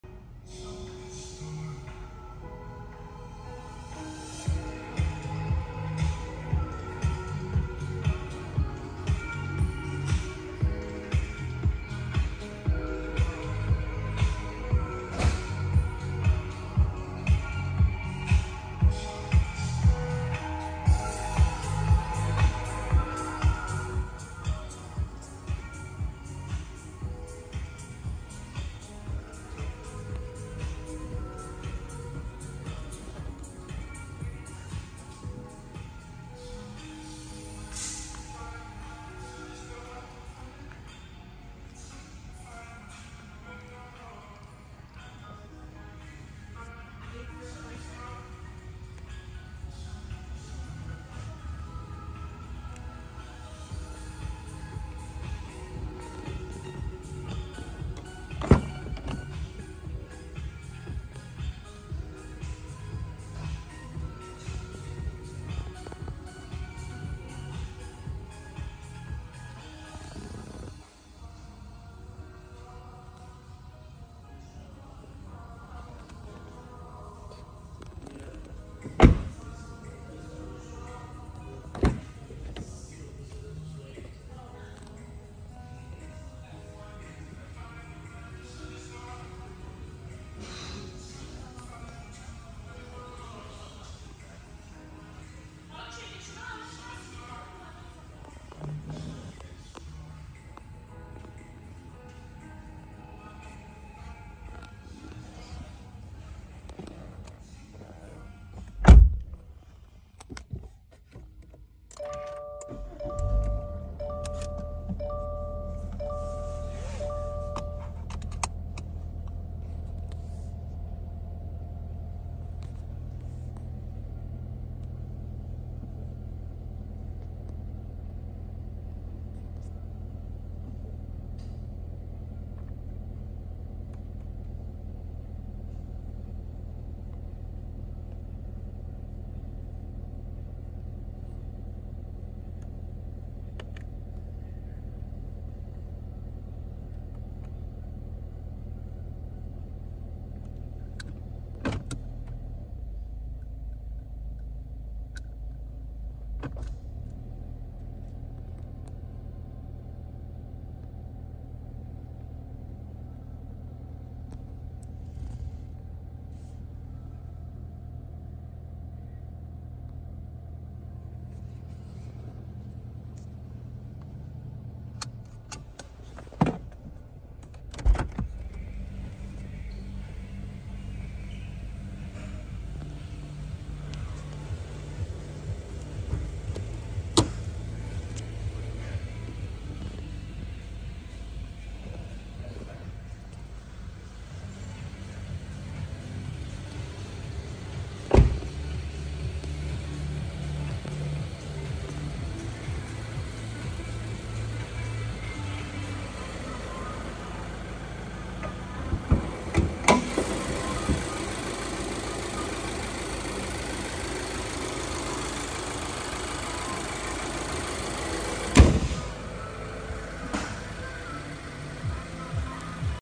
2021 Subaru Impreza Premium AWD Wagon One Owner EyeSight Driver Assist 2.0L H4 CVT - For Sale
Have a look at this One Owner, Serviced, Extra Clean, & Non Smoker 2021 Subaru Impreza Premium AWD Wagon with Silver Exterior Paint on Black Cloth Interior Upholstery. This Super Nice Subaru is equipped with a 152 Horsepower 2.0L H4 Engine Coupled with CVT Automatic Transmission. @TXAutogroup